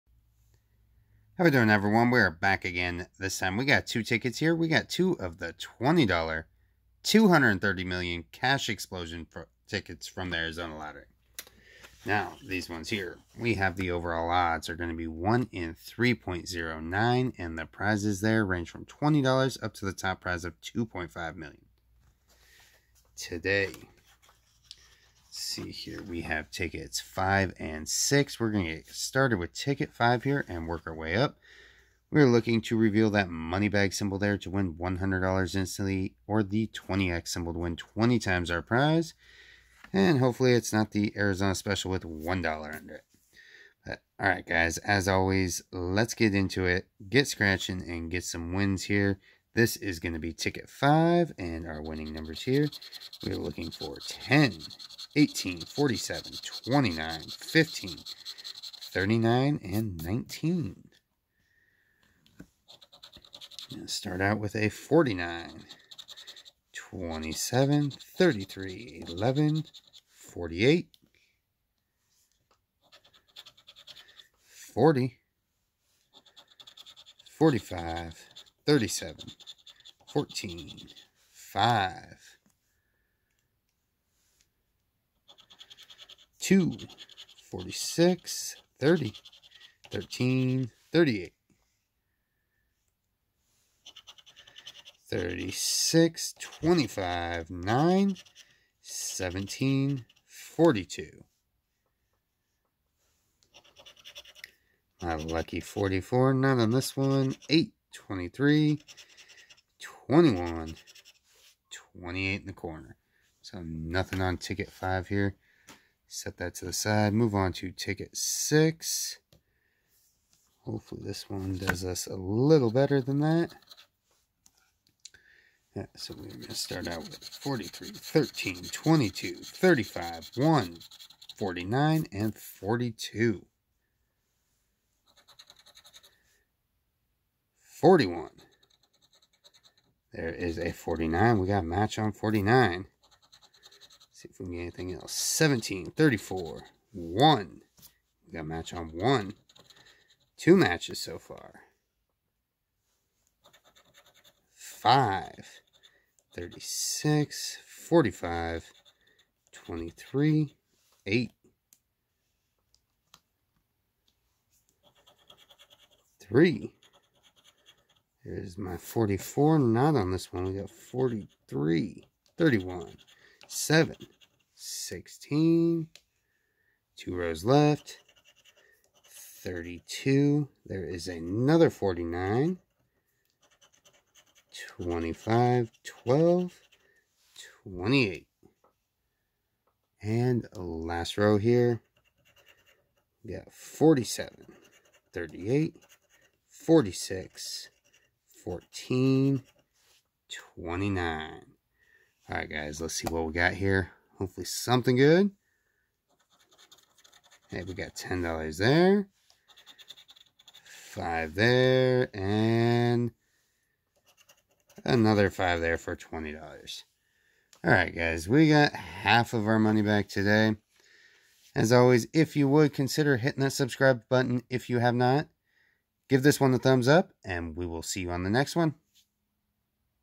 ARIZONA LOTTERY-- $230 MILLION CASH EXPLOSION!
Arizona lottery- AZ lottery scratchers

2 × $20 - $230 Million Cash Explosion tickets #1400